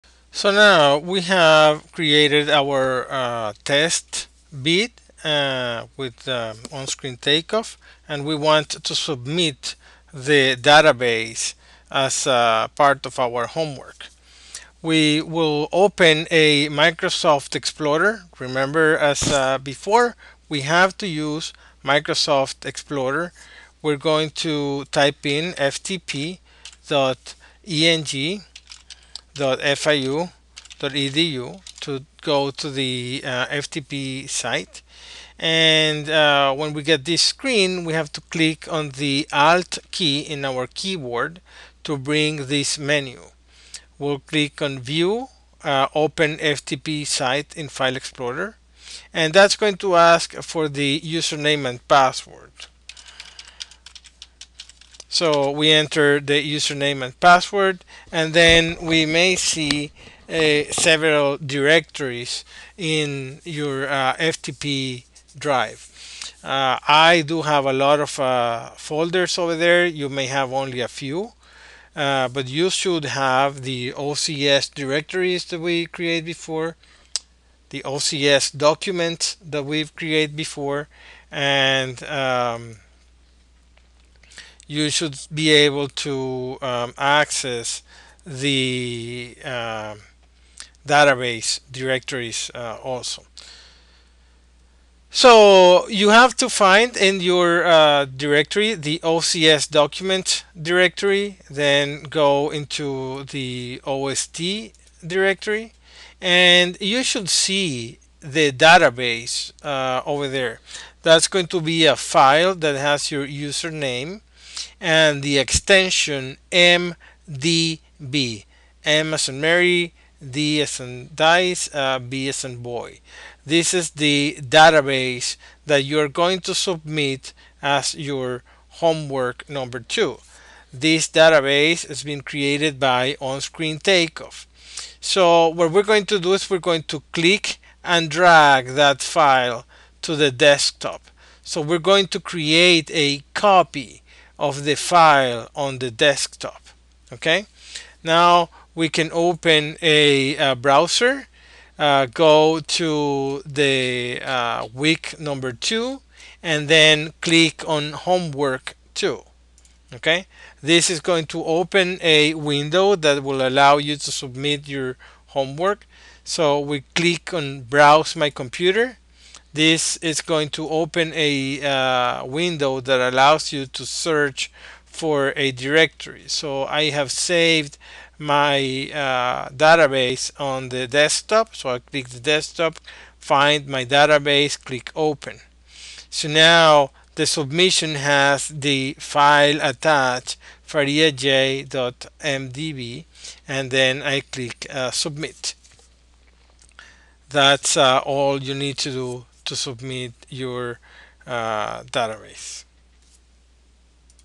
Submit your OST Database
This video shows how to make a copy of your database from the h drive to your PC desktop and submit it as your homework